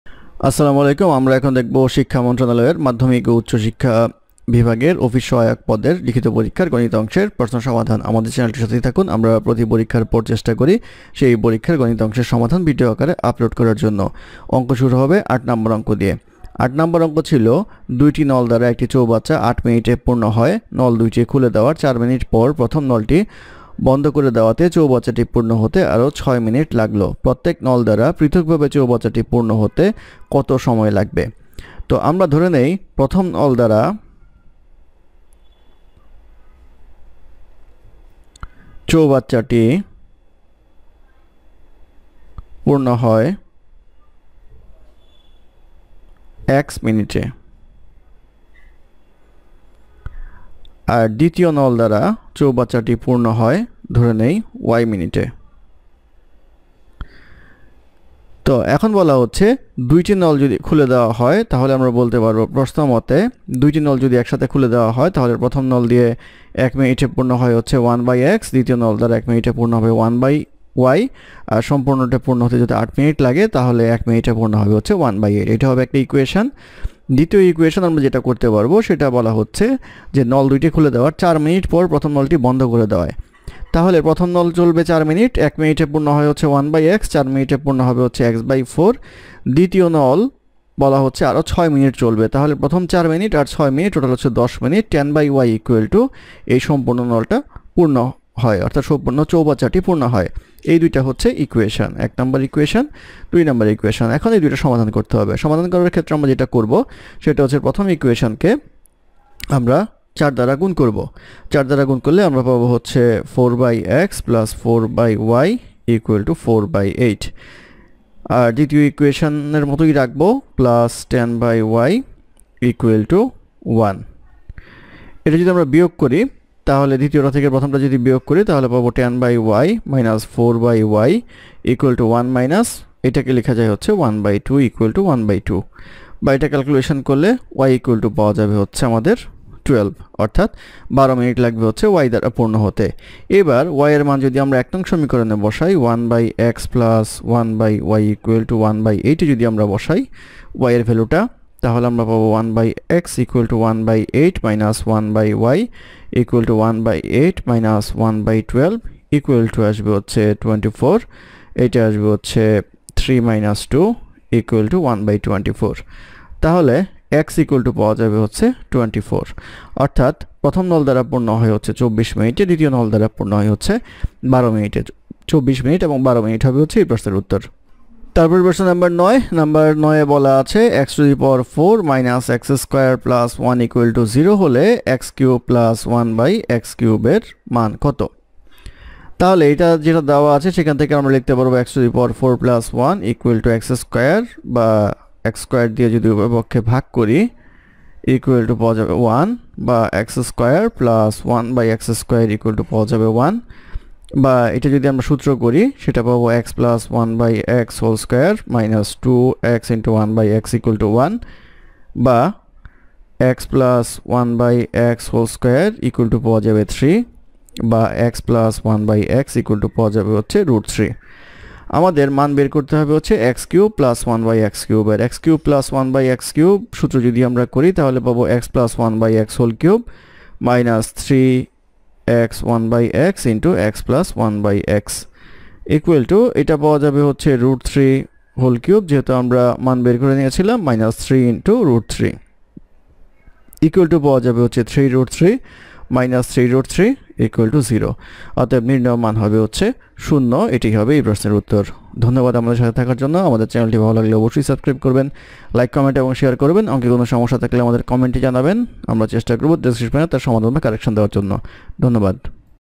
শিক্ষা মন্ত্রনালয়, মাধ্যমিক ও উচ্চশিক্ষা বিভাগ, পদঃ অফিস সহায়ক Written Math Exam Date: 12-03-2021
mathhelpline mathhelpline শিক্ষা মন্ত্রনালয়, মাধ্যমিক ও উচ্চশিক্ষা বিভাগ,
পদঃ অফিস সহায়ক

৮. দুইটি নল দ্বারা একটি চৌবাচ্চা ৮ মিনিটে পূর্ণ হয়। নল দুইটি খুলে দেয়ার ৪ মিনিট পর প্রথম নলটি বন্ধ করে দেয়াতে চৌবাচ্চাটি পূর্ন হতে আরো ৬ মিনিট লাগল। প্রত্যেক নল দ্বারা পৃথকভাবে চৌবাচ্চাটি পূর্ণ হতে কত সময় লাগবে?
৯.  x4-x2+1=0 হলে x3+ 1/x3 =?